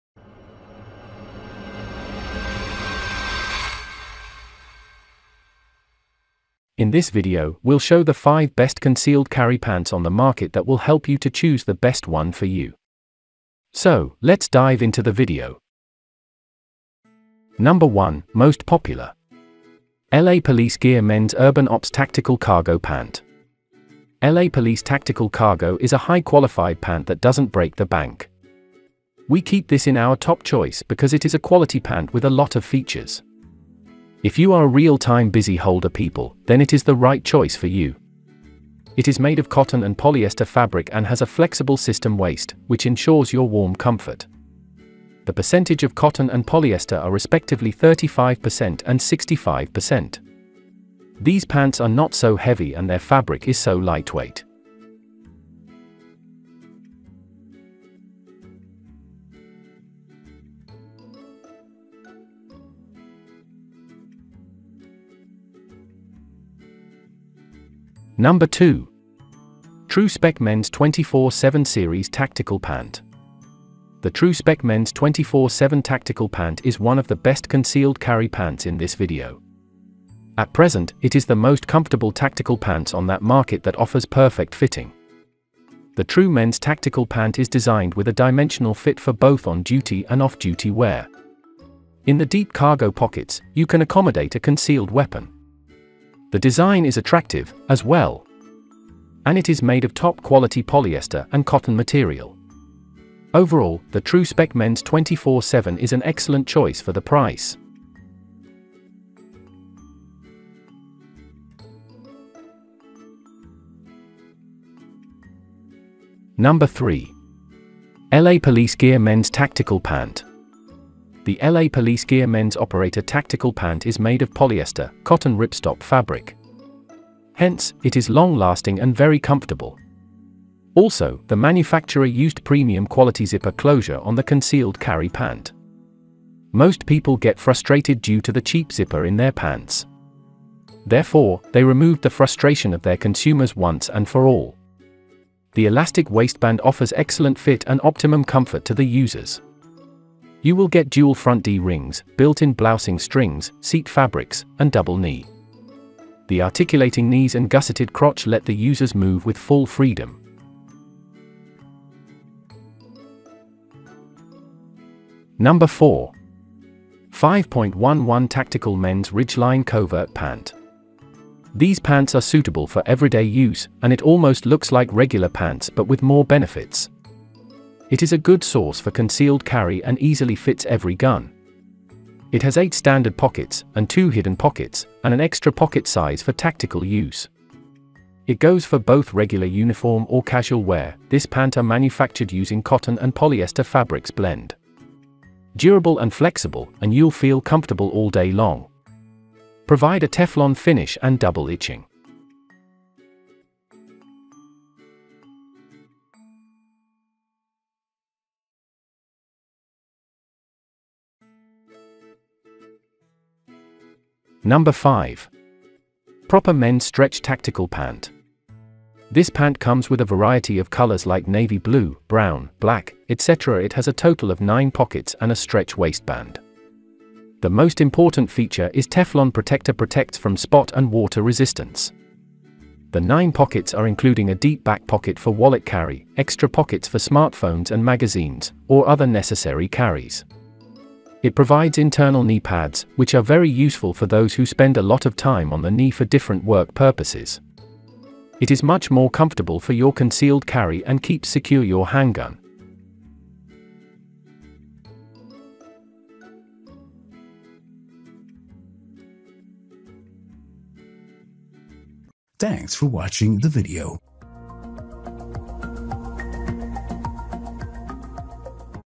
Top 5 Best Concealed Carry Pants Review in 2023 | Men’s Tactical Cargo Pants
Here is the list of five best concealed carry pants:
1. LA Police Gear Men's Urban Ops Tactical Cargo Pant
2. Tru-Spec Men's 24-7 Series Tactical Pant
3. LA Police Gear Men's Tactical Pant
4. 5.11 Tactical Men’s Ridgeline Covert Pant
5. Propper Men's Stretch Tactical Pant

🕕Timestamps🕕
0:00 - Introduction
0:20 -LA Police Gear Men's Urban Ops Tactical Cargo Pant
1:10 -Tru-Spec Men's 24-7 Series Tactical Pant
1:59 -LA Police Gear Men's Tactical Pant
2:48 -5.11 Tactical Men’s Ridgeline Covert Pant

3:36 -Propper Men's Stretch Tactical Pant

🤨 What is a concealed carry pant?
Concealed Carry Cargo Pants are the best tactical pants available because they conceal and carry larger firearms, and carry more spare magazines than all the competitors.